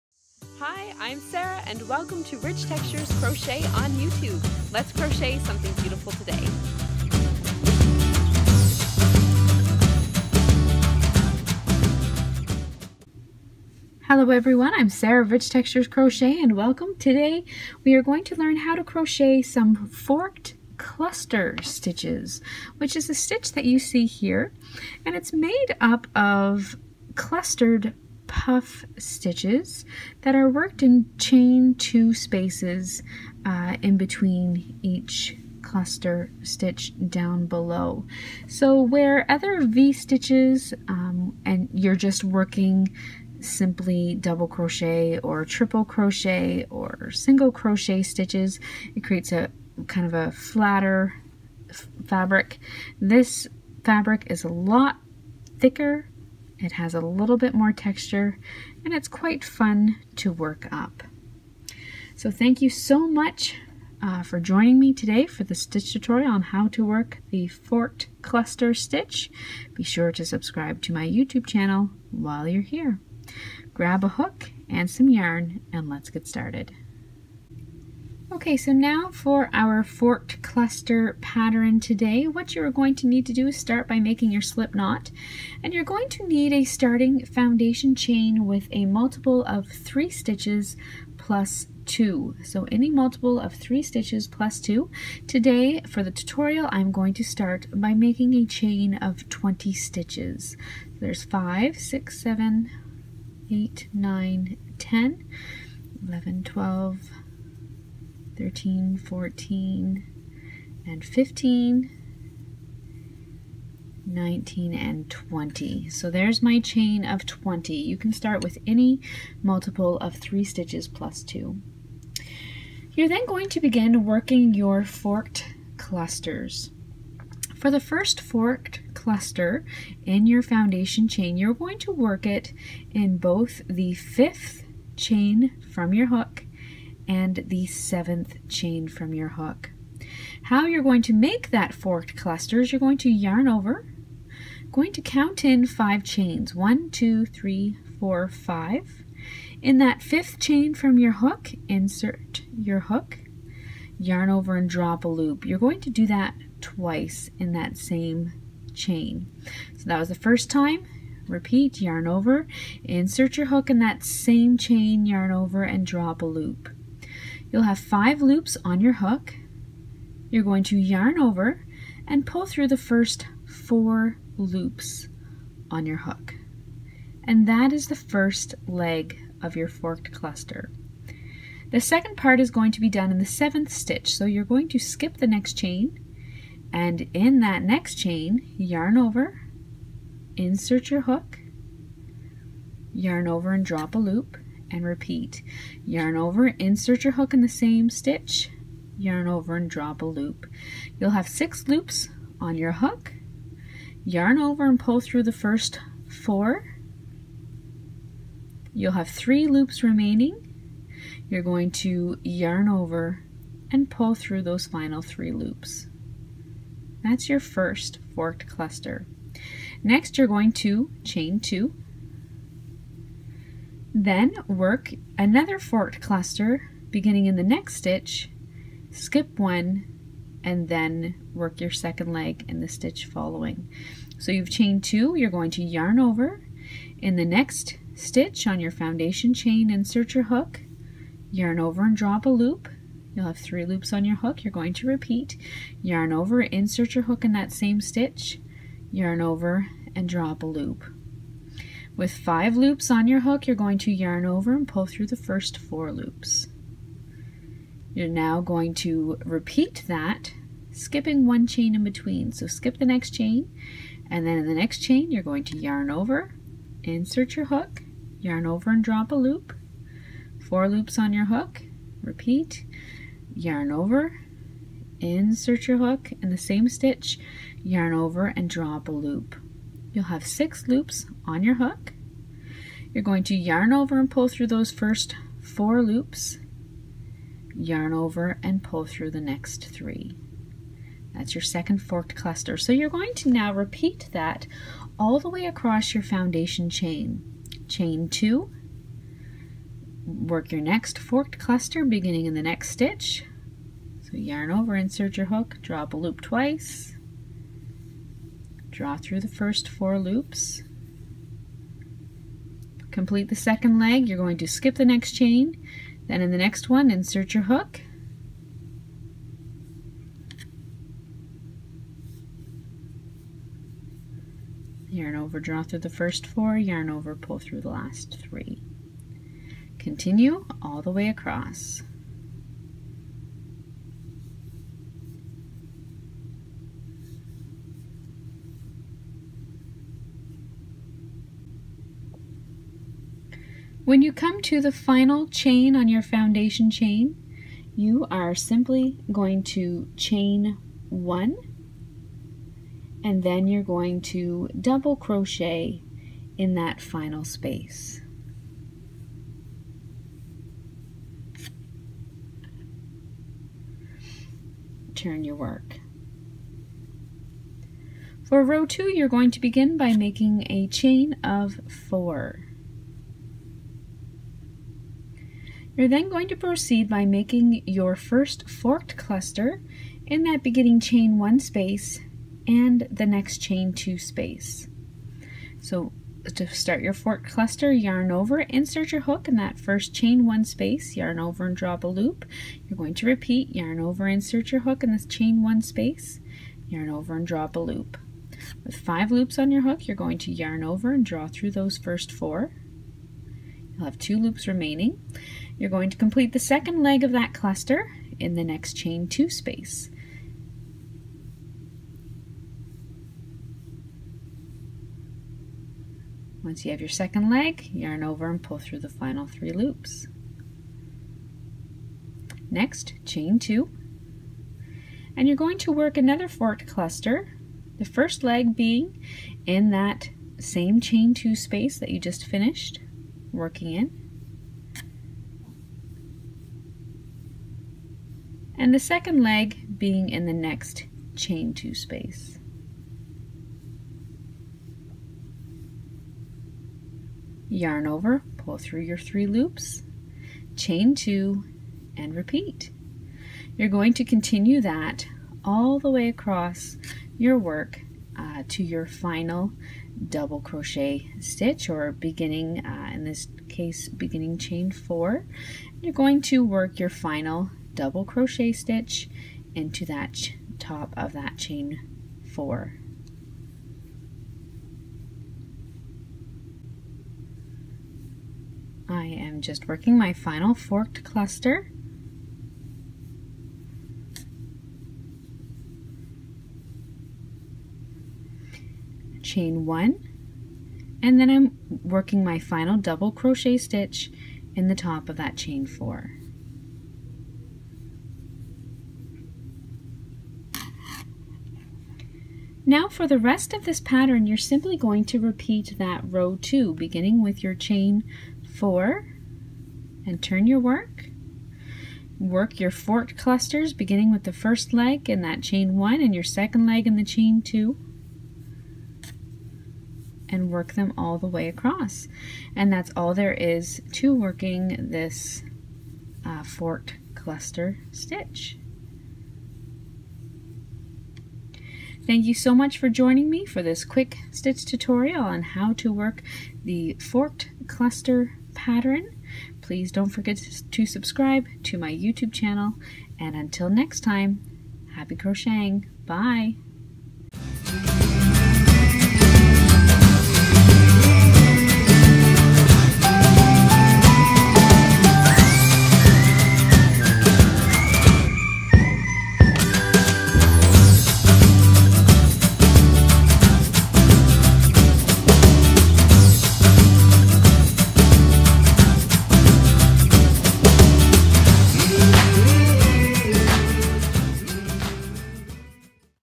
Forked Cluster Stitch - How to Crochet
Today we are going to learn how to crochet the Forked Cluster Stitch.

Sarah